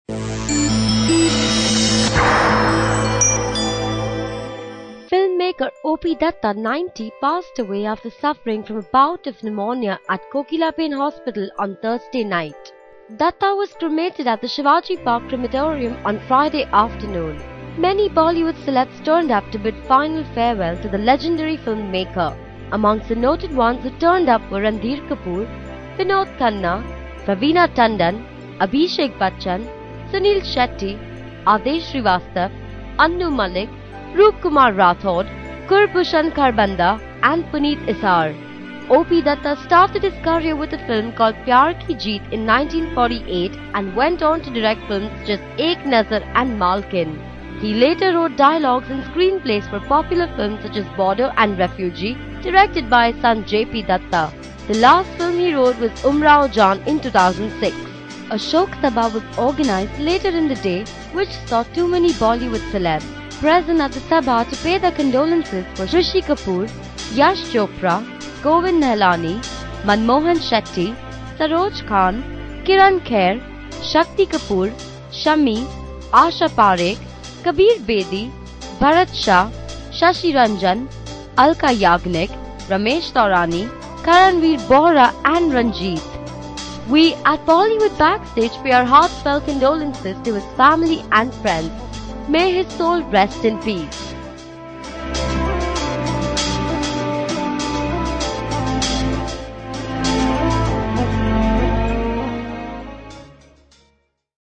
Bollywood pays tribute to late filmmaker O.P.Dutta
Many Bollywood celebs turned up to bid final farewell to the legendary filmmaker O.P.Dutta. He passed away after suffering from a bout of pneumonia at Kokilaben Hospital on Thursday night..